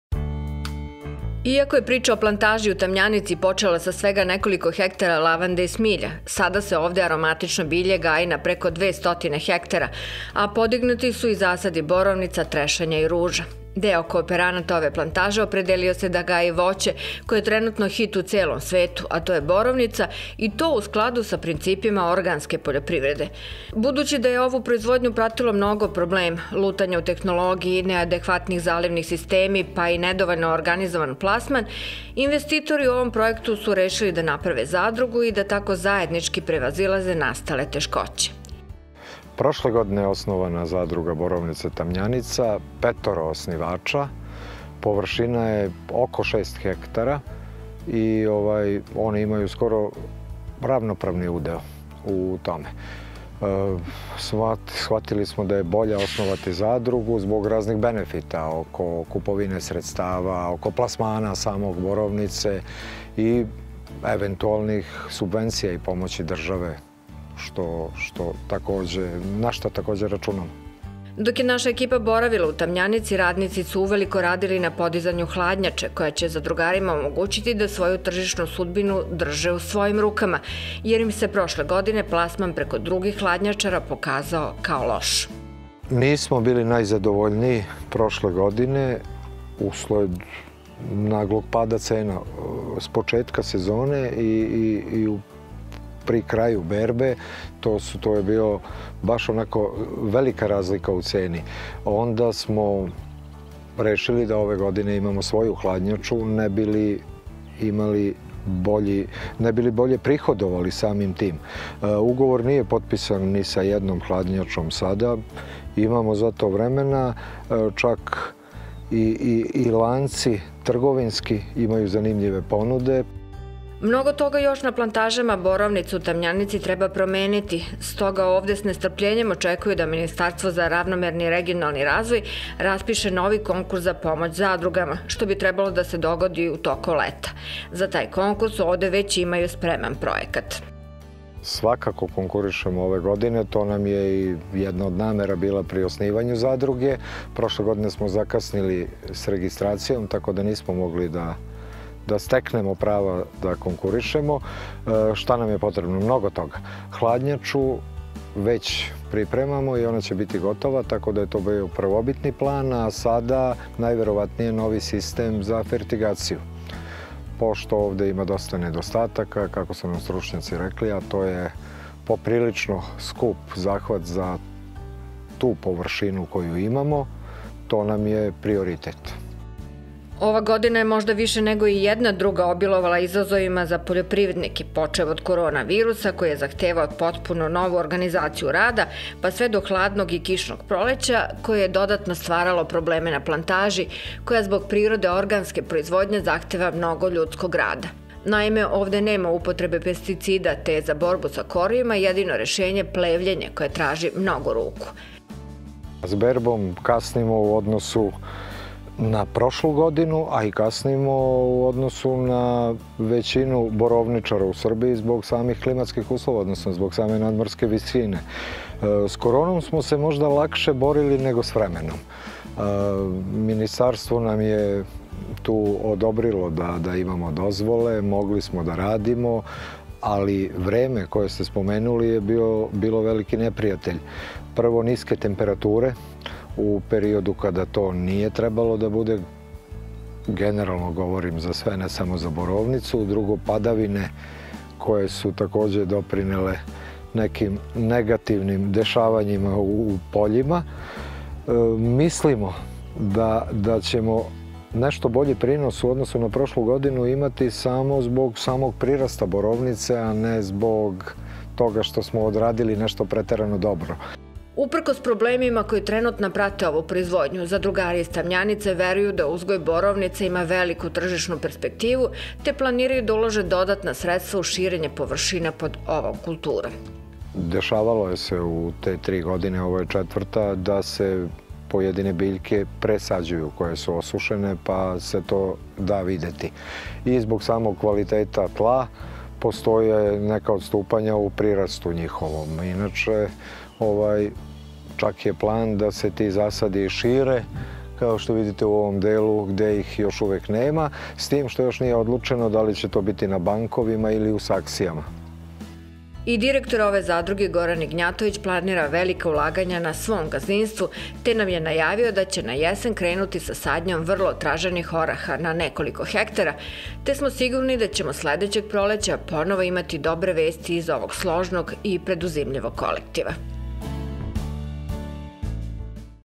ZADRUGA za USPEŠNIJU PROIZVODNJU BOROVNICA
U kojoj meri će novoosnovana zadruga proizvodjača borovnice na jugu Srbije unaprediti  poslovanje tamošnjih poljoprivrednika?
Sagovornik: Goran Ignjatović - direktor ZZ Borovnica, Tamnjanica
Facebook : Farma emisija o poljoprivredi